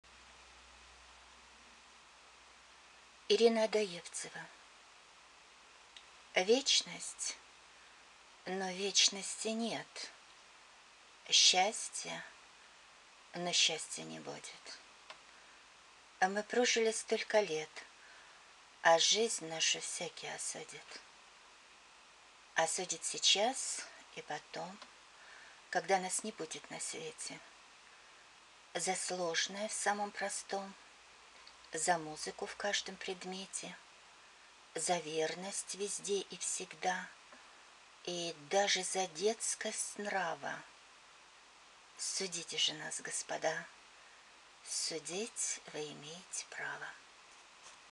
Ирина Одоевцева. Вечность?...
Вечность? Но вечности нет.
Счастье? Но счастья не будет.
Мы прожили столько лет,
А жизнь нашу всякий осудит.

Осудит сейчас и потом,
Когда нас не будет на свете.
За сложное в самом простом,
За музыку в каждом предмете,
За верность везде и всегда
И даже за детскость нрава.

– Судите же нас, господа.
Судить вы имеете право.